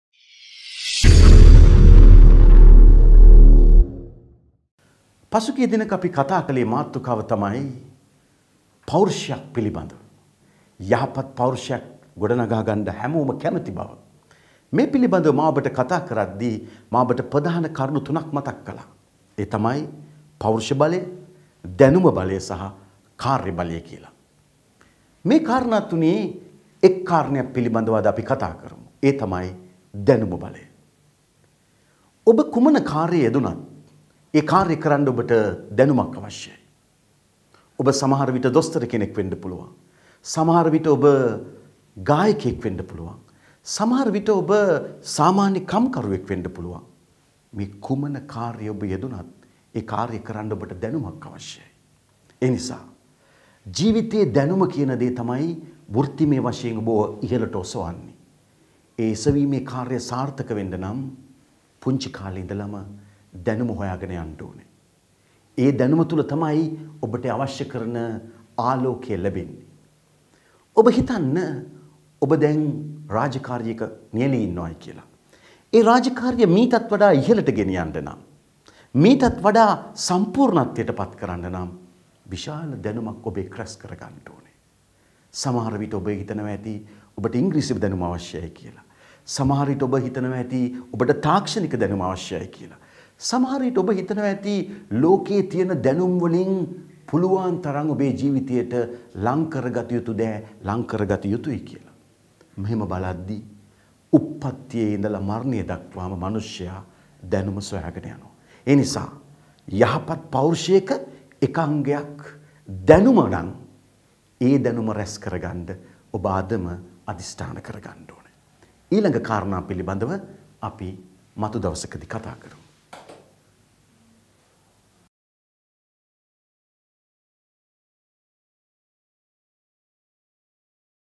පෞර්ෂත්වය - 2 (අභියෝගය බාරගනිමු!) | Personality - 2 (Challenge Accepted!)
චන්දන තිසේරා මහතා ඉදිරිපත් කරන, අභියෝගය බාරගනිමු කතා මාලාව ඔස්සේ දිගහැරෙන නවතම මාතෘකාවයි, පෞර්ෂය.

පුද්ගලයන් හඳුනාගන්න, විවිධ වූ පෞර්ෂයන් හඳුනාගන්න,  පෞර්ෂ බලය, කාර්‍ය බලය, දැනුම බලය ගැන දැනගන්න, ඔබේ පෞර්ෂය වර්ධනය කරගැනීමට කළ යුතු දේ ගැන අවබෝධයක් ලබාගැනීමට, මෙම වීඩියෝ මාලාව නරඹන්න.